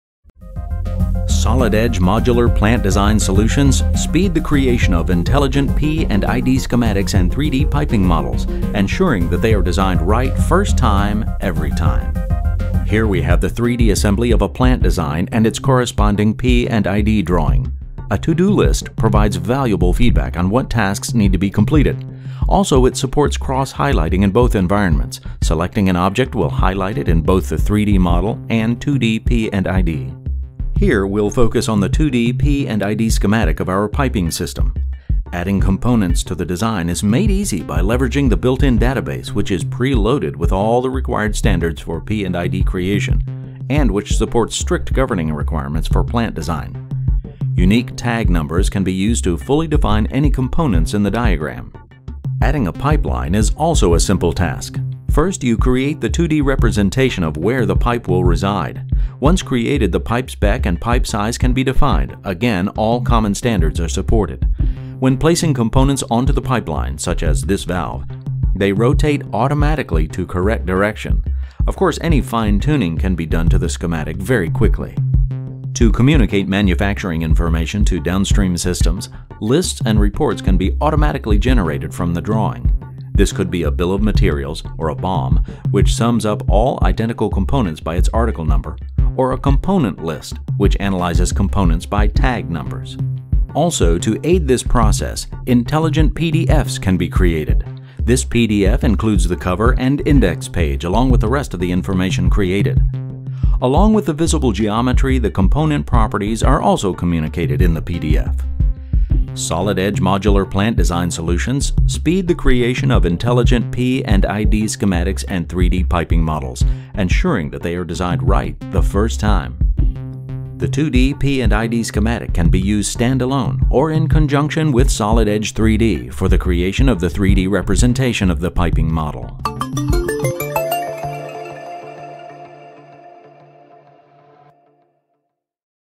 Solid Edge P&ID and Piping Design
Create P&ID's by providing 2D flow diagram and symbol support and easily place them into a 3D model to provide a complete modular plant design solution. Solid Edge MPD creates intelligent PDF's in which component properties can also be communicated for the visible parts in the PDF.